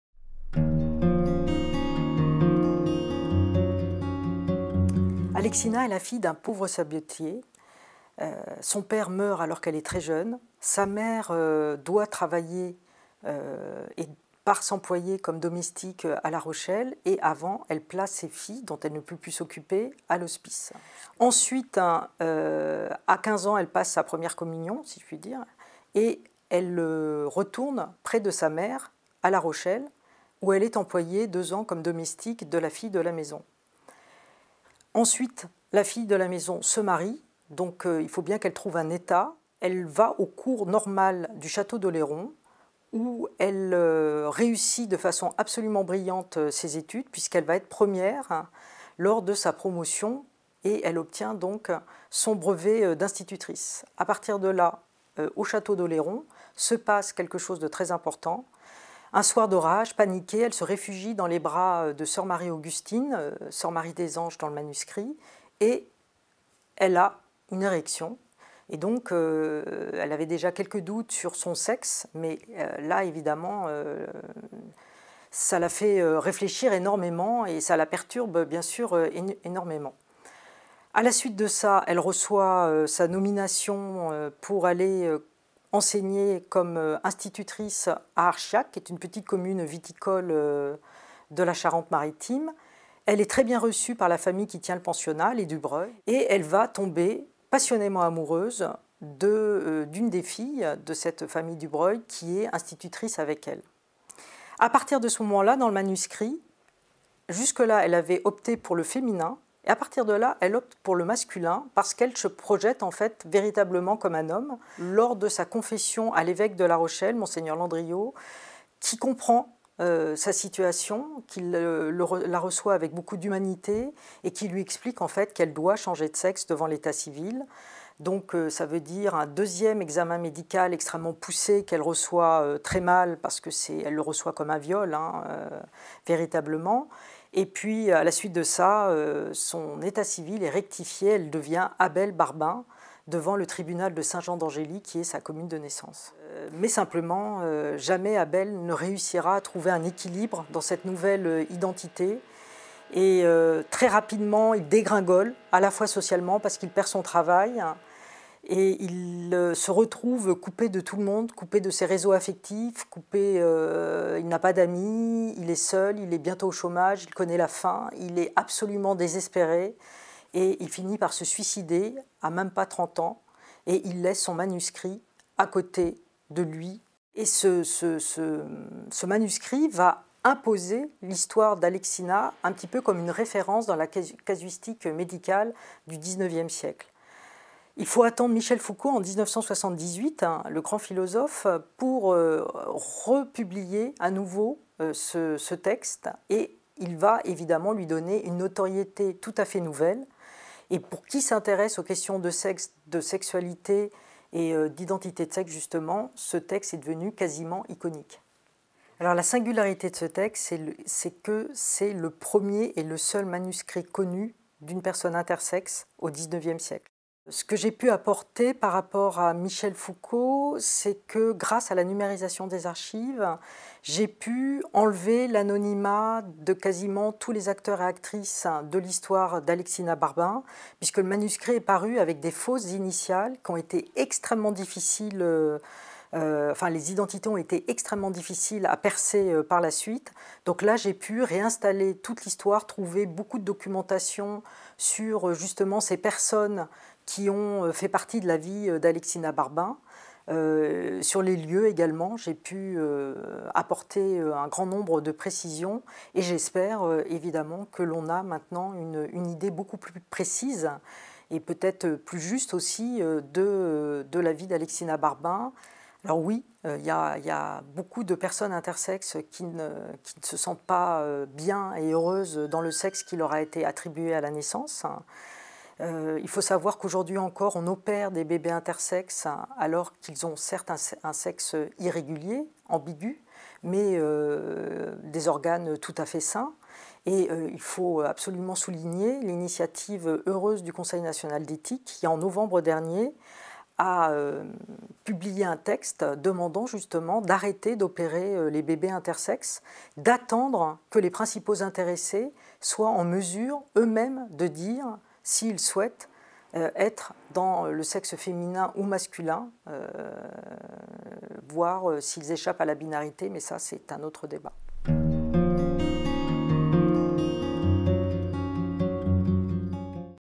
Gabrielle Houbre - Les deux vies d'Abel Barbin
Gabrielle Houbre vous présente son ouvrage "Les deux vies d'Abel Barbin, né Adélaïde Herculine (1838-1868) : édition annotée des Souvenirs d'Alexina Barbin" aux éditions PUF.